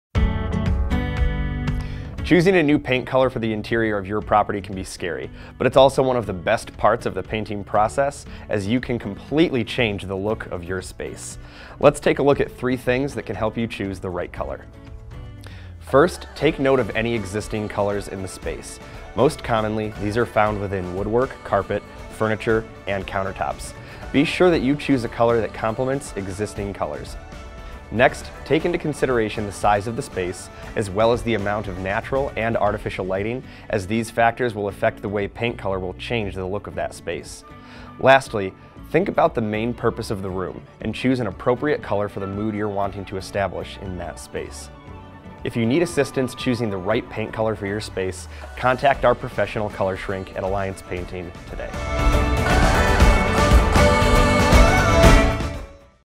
How To Choose The Right Paint Color For Your Space | Alliance Painting
Trying to find the perfect paint color for the interior of your home or business? Our ColorShrink at Alliance Painting is here to help! today!

In today’s video, we cover the basics of how to choose the right paint color for your space. It is important when choosing a color that you take into consideration the other colors that exist in the space, such as those in the furniture, woodwork, carpet, or countertops. Additionally, consider the size of the space as well as the main purpose of it. Our Alliance Painting ColorShrink is here to assist you in choosing the perfect color for your space. Get in touch today to get started!